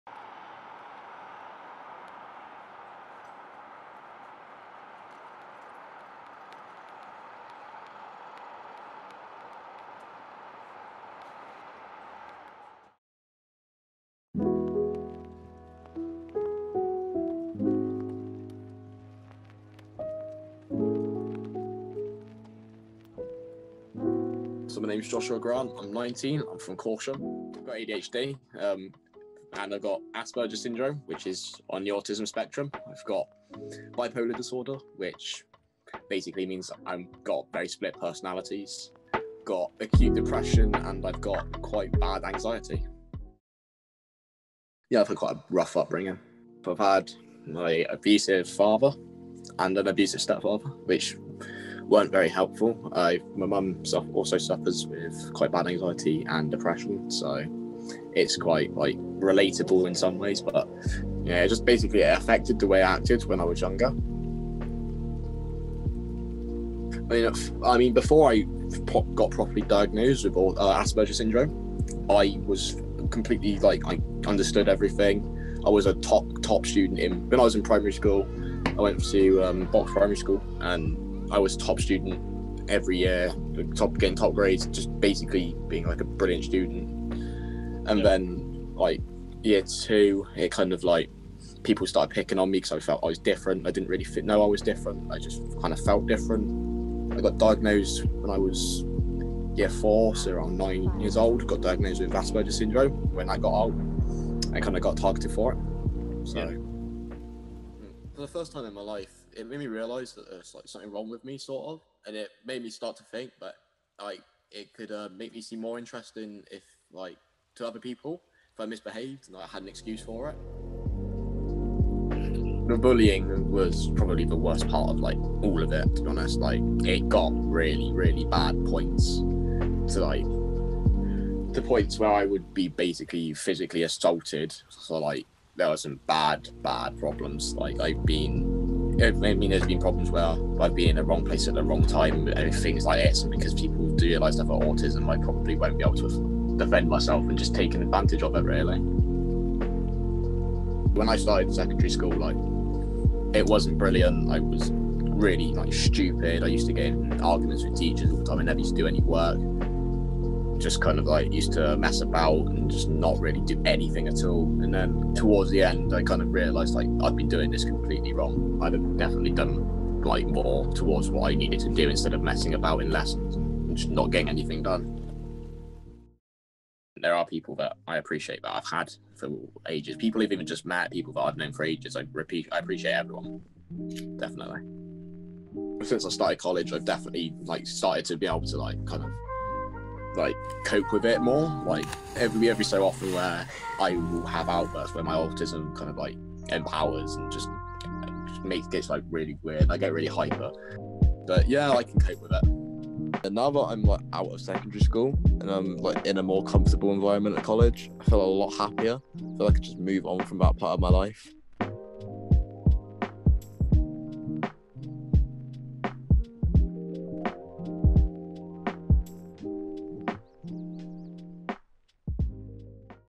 LIFE OF JOSH - A DOCUMENTARY SHORT
This is a documentary short based on my friend Josh and his life with Aspergers syndrome.